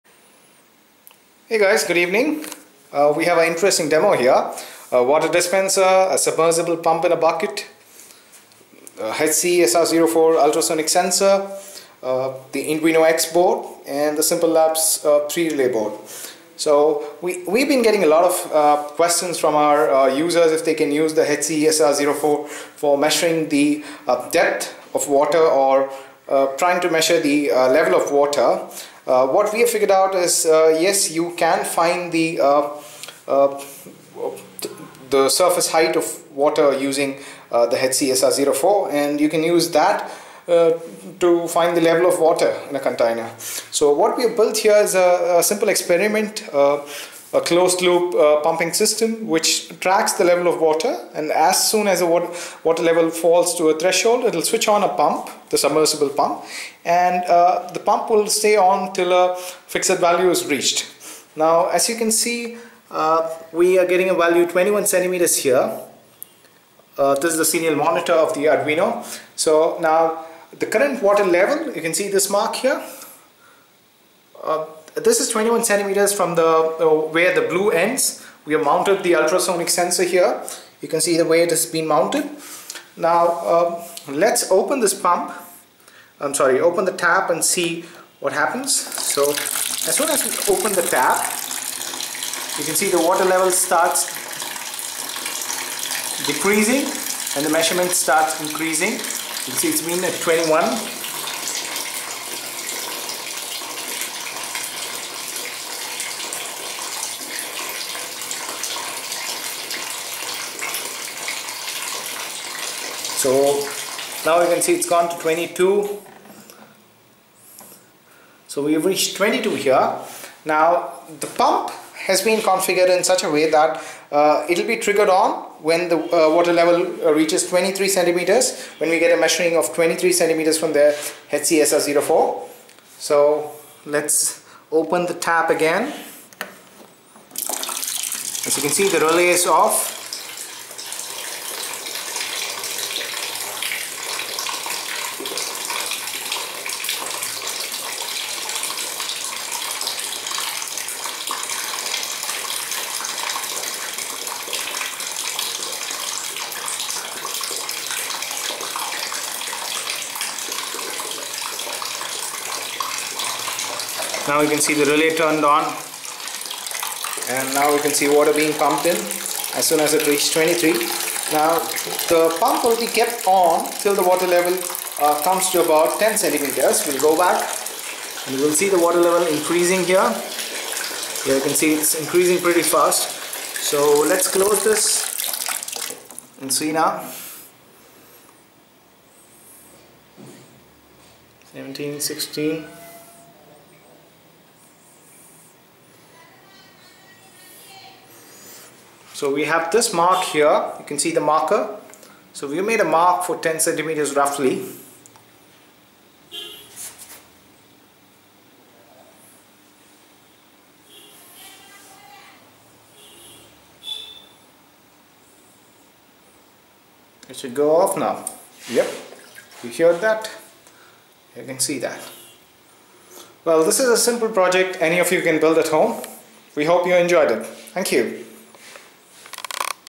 Simple Water Level Controller
A Simple Water Level Controller Demo Using A Submersible Fish Tank Pump, InduinoX Board, Simple Labs 3-Relay Board and HCSR-04

2021 Update
Learn to Build IoT Project Hands-On with our All New Program
Project Internship Program in Internet of Things